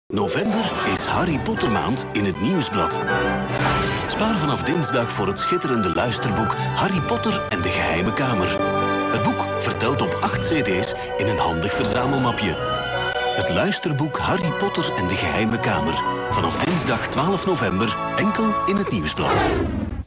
Harry Potter luisterboek Commercial 2002 (Dutch).
Harry Potter en de Geheime Kamer Luisterboek (2001).
Gratis bij Het Nieuwsblad (Flemish).